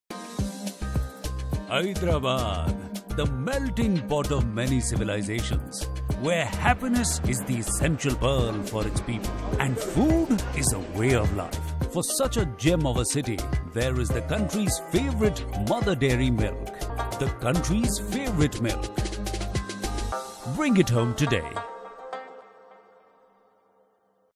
Mother Dairy Milk for the city that loves food, Hyderabad.
For the city, where food is a way of life, Mother Dairy Milk is the perfect partner who caters to all the needs of the family. Every drop of Mother Dairy Milk goes through 29 stringent quality checks to give you the guarantee of 100% quality. So Hyderabad, bring home the country’s favourite Mother Dairy Milk today.